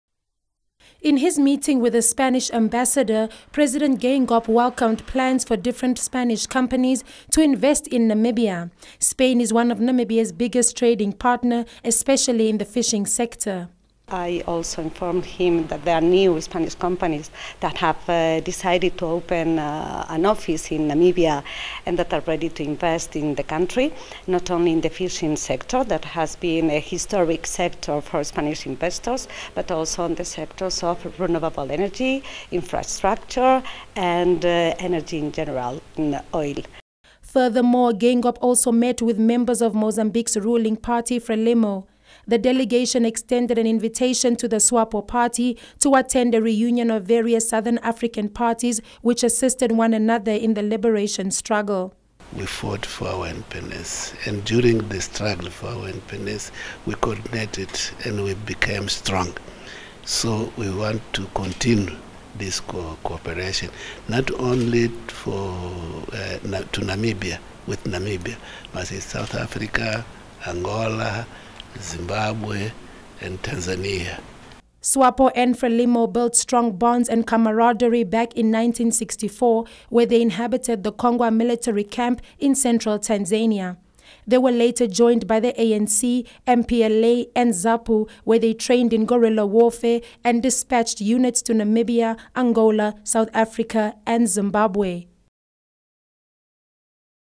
President Geingob and Ambassador to Kingdom of Spain, Carmen Diaz solidify relations
by: Grace Luvindao ---
Earlier in the day, President Hage Geingob met with the Ambassador of the Kingdom of Spain Carmen Diaz to cement relations between the two nations.